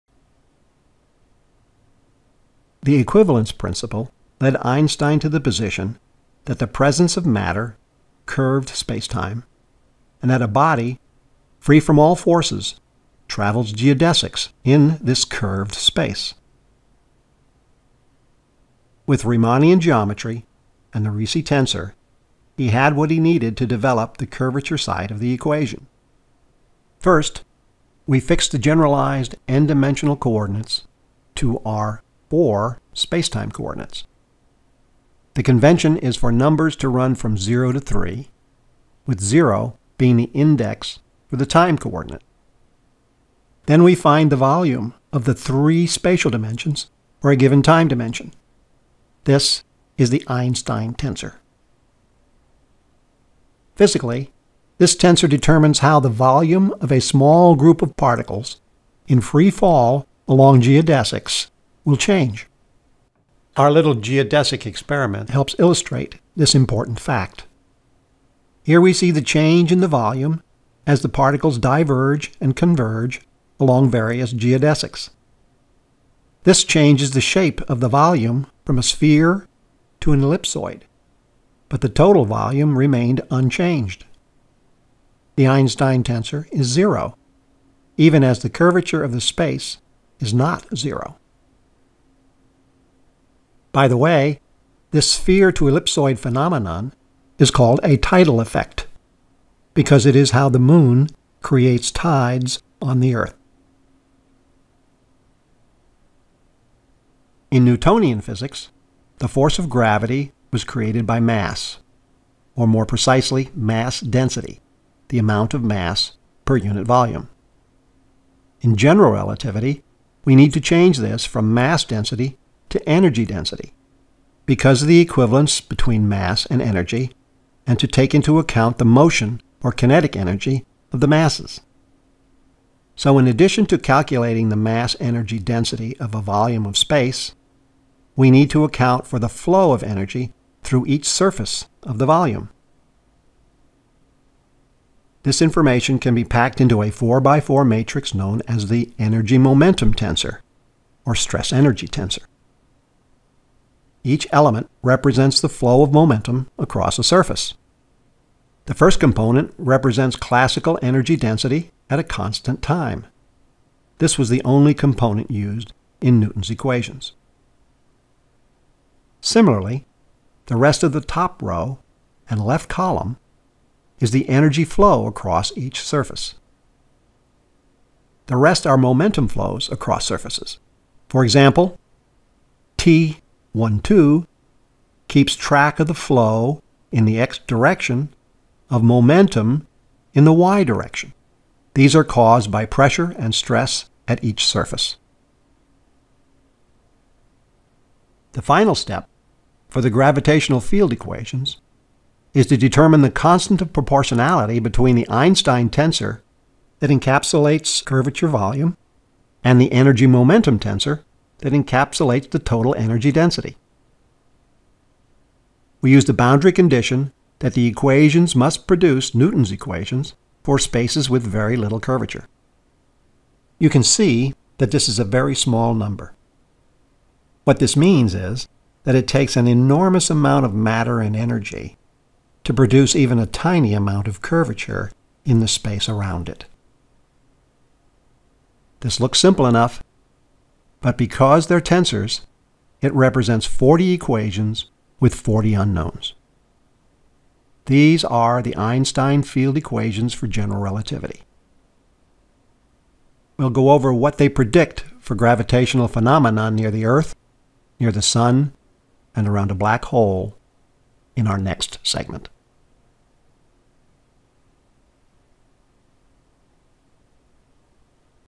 Classroom Aid - Einstein Field Equations
In this segment of the “How Fast Is It” video book, we cover the geometry of general relativity. We start with the Elevator Thought Experiment, and show how it represents a gravitational field and how it predicts the bending of light. This sets the stage for the Equivalence Principle. This leads to the reconciliation of Newton’s two definitions for mass. Which, in turn, leads to the idea that the existence of a mass bends space. To understand the bending of space, we cover the basics of Euclidian and non-Euclidian Riemann geometry. We include spherical and hyperbolic geometries along with the nature of their respective geodesics. We actually measure geodesic deviation above the Earth. For a fuller understanding, we cover the definition of metrics and curvature in terms of tensors. With the general Riemannian Curvature Tensor in hand, we find the subsets that reflect the behavior of space within a volume. We then cover how Einstein mapped this geometry to space-time to produce the Einstein Curvature Tensor. And finally, we describe the Energy-Momentum tensor that identifies the nature of a volume of matter-energy, which is the source of the space-time curvature. Setting these equal to each other with an appropriate conversion factor gives us Einstein’s general relativity field equations.